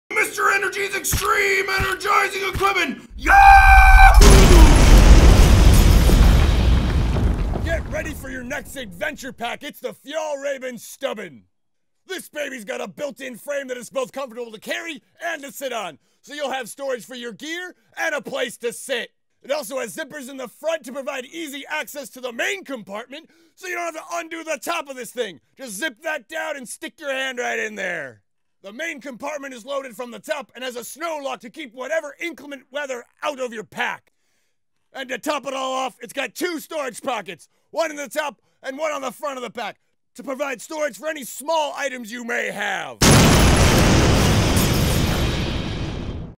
Fjallraven Stubben Backpack 2017 Review
A hybrid between a rugged daypack and a camp stool, the Fjallraven Stubben Backpack is a great solution for hunting, ice fishing, or any short-term foray into the great outdoors. With an internal steel frame and padded top, this pack sits upright to create a comfy seat. There's even an integrated waterproof seat cover in case inclement weather strikes. Featuring burly G-1000 fabric treated with Greenland Wax, this bag is crafted for long-term weather resistance and durability. Front zip access to the main compartment offers a convenient way to access pack contents without unpacking, shuffling through the bag, or leaving your seat.

FEATURES
- Built-in frame that is comfortable both to carry and to sit on
- Zippers in the front provide easy access to the main compartment without needing to unbuckle the top lid
- The main compartment is loaded from the top, has a snow lock and is lined with orange nylon
- Top lid is attached to the front with leather straps
- A pocket in the top lid and in the bottom front provide storage for both small items and larger items
- The seat and support system rest on an ergonomically designed aluminum frame